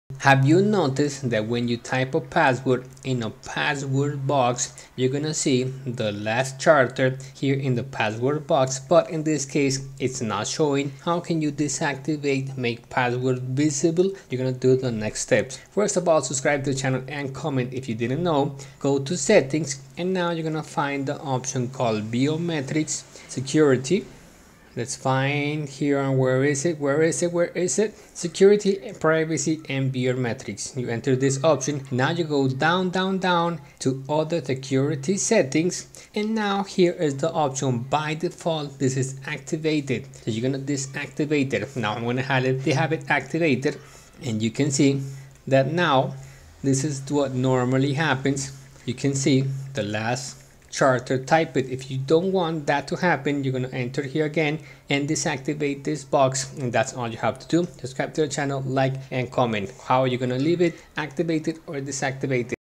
how to make password invisible in password box on my Samsung A54 5G, A53, A73, S23 Ultra
In this tutorial video, we will show you how to enhance the security and privacy of your Samsung A54 5G, A53, A73, or S23 Ultra smartphone by making the password invisible in the password box. When you enter a password in a password field, it is usually displayed as asterisks or dots for security reasons, so that no one can easily see your password. However, some users may prefer to keep their password completely hidden from prying eyes or avoid accidental shoulder surfing. We will guide you through the step-by-step process of making your password invisible in the password box, giving you peace of mind and an extra layer of protection.

Step 1 - Accessing "Settings": Configuring Your Device
The first step is to access the settings menu on your Samsung device, where you can find various device preferences and configurations.

Step 2 - Navigating to "Biometrics and Security": Enhancing Security Settings
Within the settings menu, we will guide you to the "Biometrics and Security" section, which houses various security-related options for your phone.

Step 3 - Selecting "Secure Lock Settings": Customizing Lock Screen Security
In this step, you'll learn how to access the "Secure Lock Settings," where you can customize the security features of your lock screen.

Step 4 - Entering Your Current Password: Authenticating Your Access
To proceed with modifying the password visibility, you will need to enter your current password to verify your access to the security settings.

Step 5 - Toggling "Password Invisible": Making Your Password Hidden
Once in the "Secure Lock Settings," we'll show you how to toggle the "Password Invisible" option to make your password completely hidden when you enter it.

Step 6 - Confirming the Changes: Ensuring Password Visibility
After making the necessary changes, we'll demonstrate how to confirm that your password is now invisible in the password box.

Step 7 - Testing the Password Field: Verifying the Result
To ensure that the password visibility has been successfully adjusted, we'll guide you through a simple test of the password field.

Step 8 - Reverting the Changes: Restoring Password Visibility
In case you want to revert to the default setting and make your password visible again, we'll provide the steps to restore password visibility.

Conclusion:
Congratulations! You have learned how to make your password invisible in the password box on your Samsung A54 5G, A53, A73, or S23 Ultra smartphone. By following our step-by-step instructions, you now have the option to enhance your device's security and privacy by keeping your password hidden from view. Whether you choose to keep your password invisible or revert to the default setting, you have gained greater control over your device's security features.